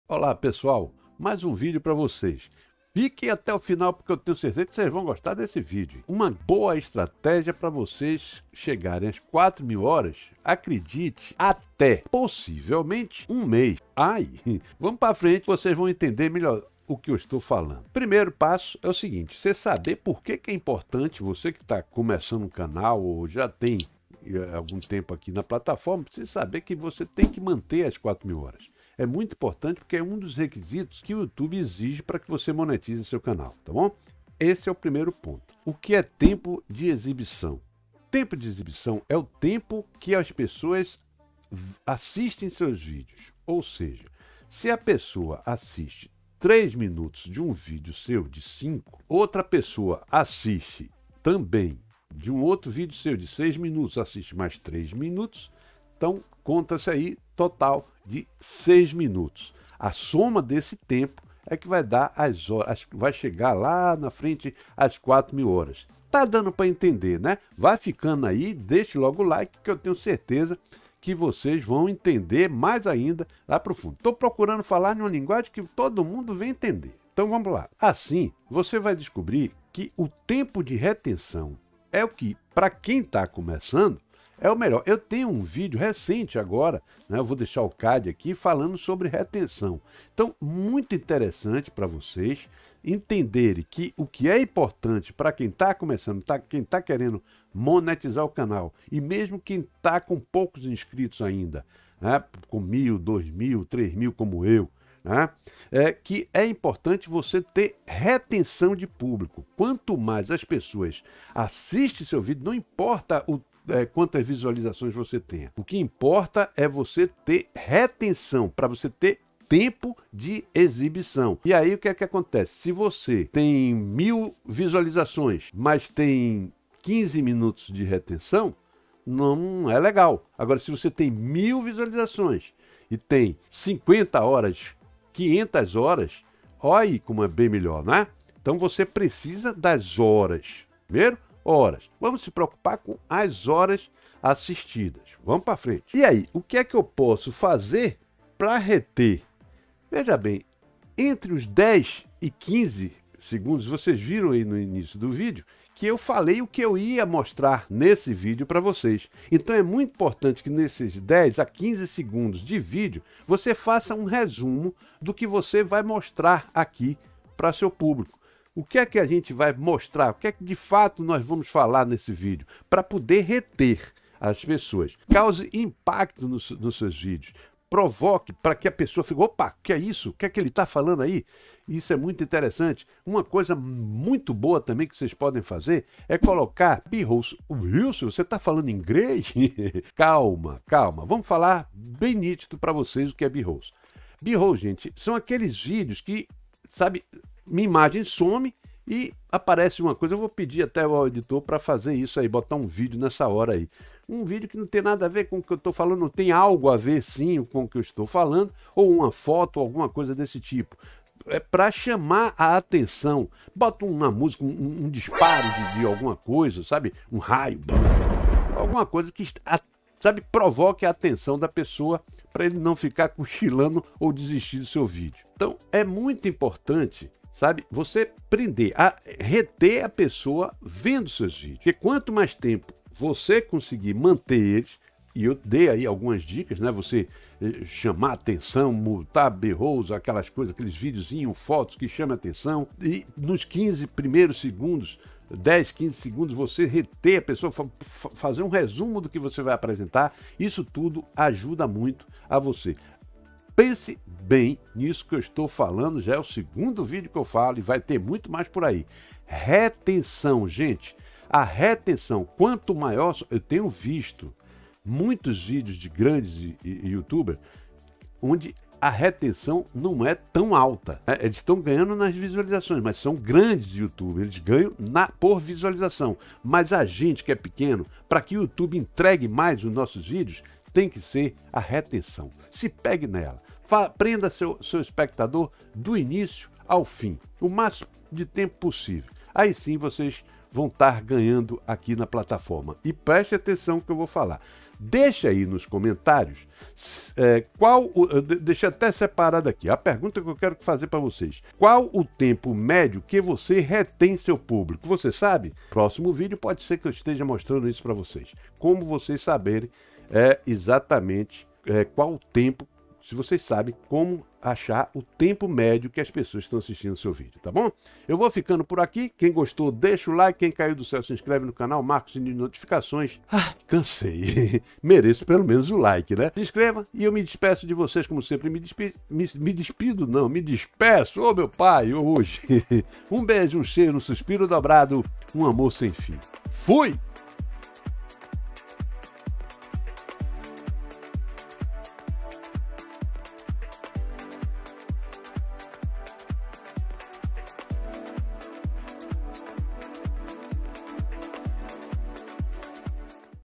ESTRATÉGIA que pode fazer VOCÊ conseguir AS 4 MIL HORAS em 1 MÊS!!!
Neste vídeo estarei mostrando uma estratégia que pode fazer com que você consiga as 4 mil horas em 1 mês!!!